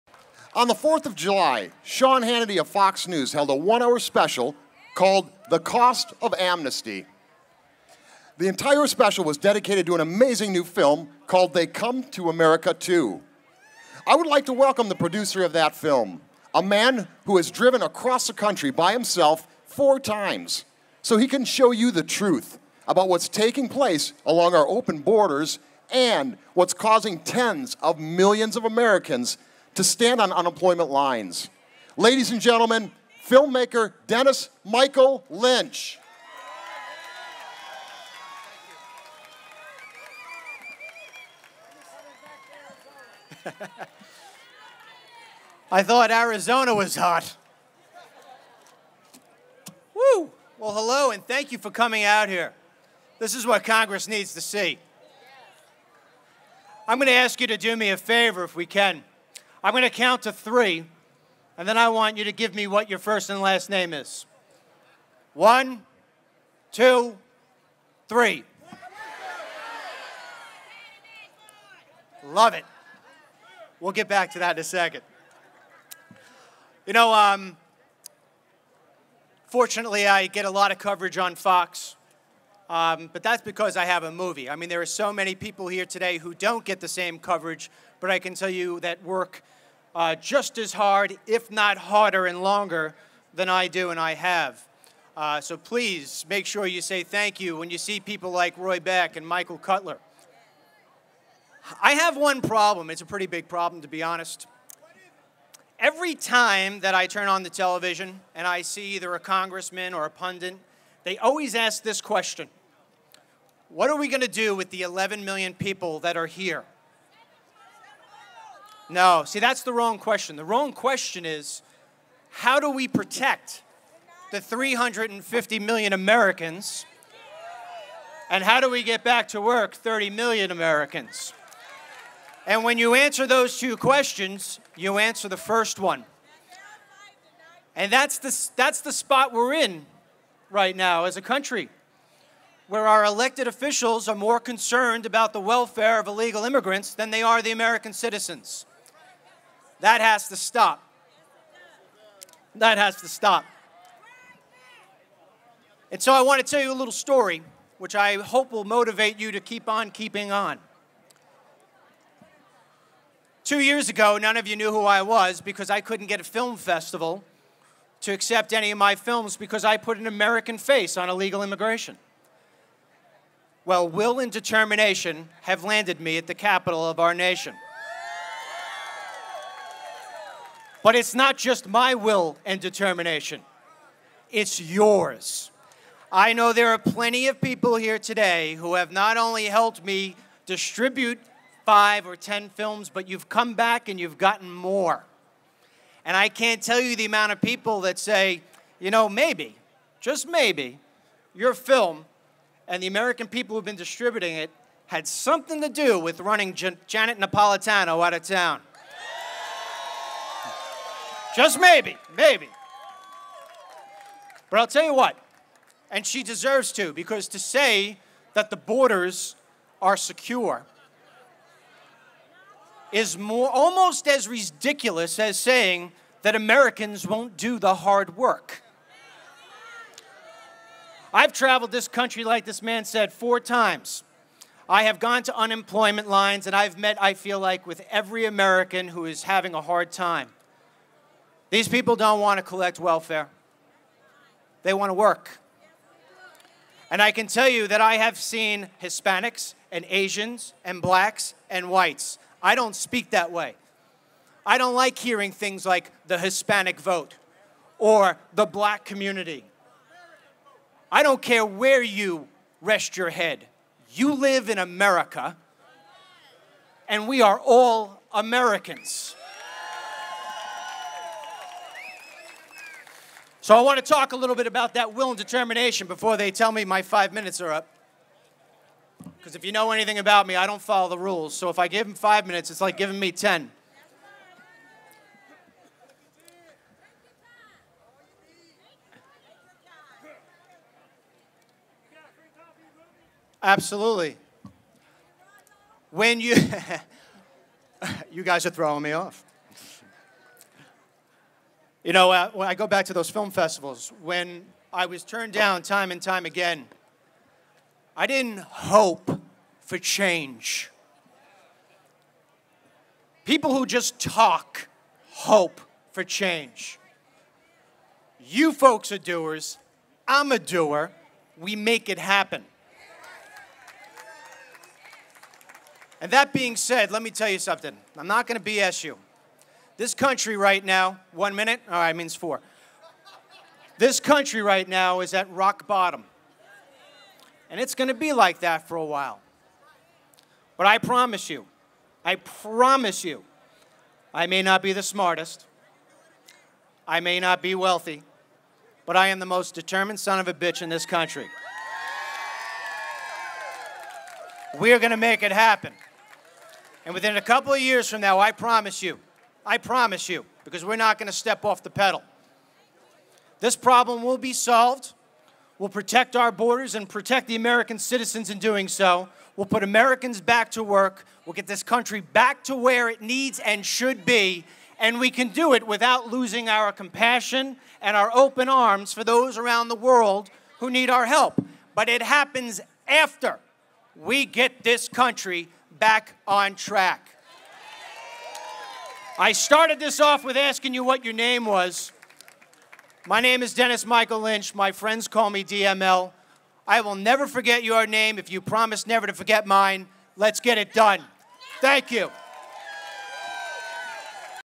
Dennis Michael Lynch at the DC March for Jobs
The Black American Leadership Alliance (BALA) organized the the DC March for Jobs, which was held on July 15, along with community leaders of all races from around the country. The march was organized in an attempt to stop Congress' amnesty push, enforce immigration laws as written, and support policies that put black U.S. citizens back to work.